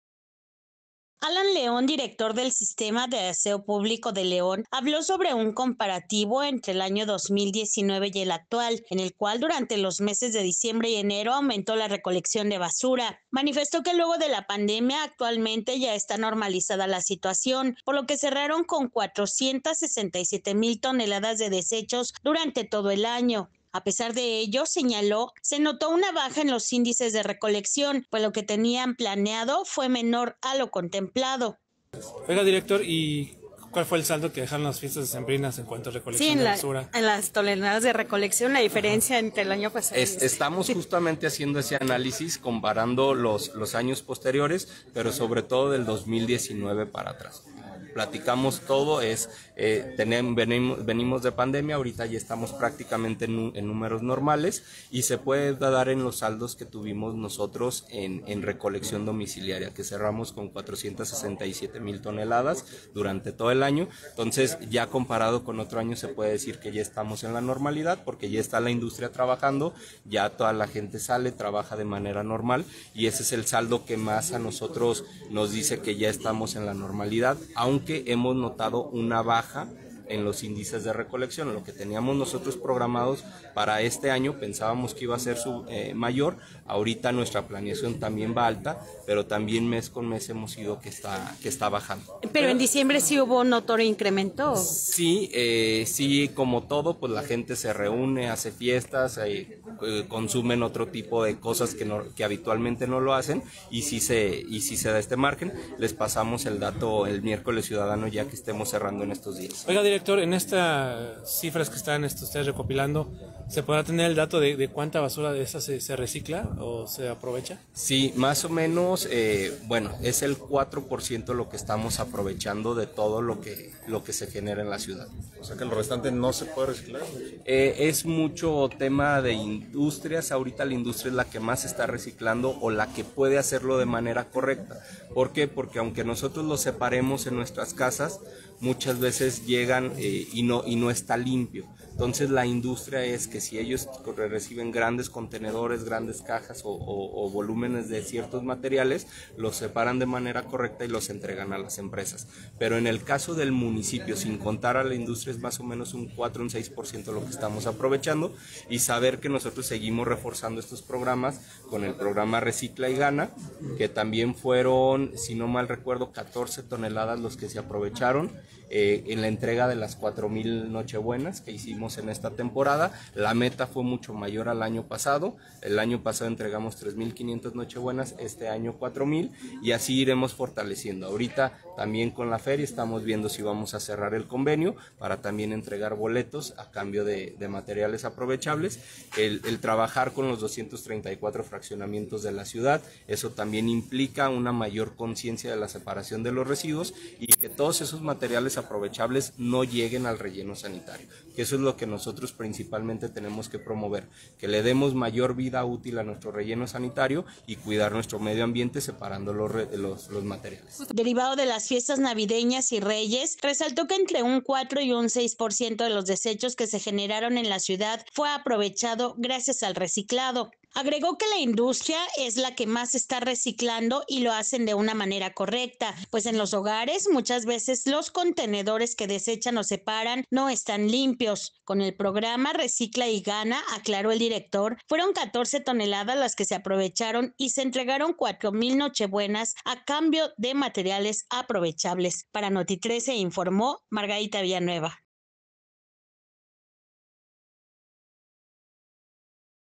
En los meses de diciembre y enero aumentó la recolección de basura en León
VideoNota🎥🔴 | Durante los meses de diciembre y enero aumentó la recolección de basura reportó el sistema de aseo público de León.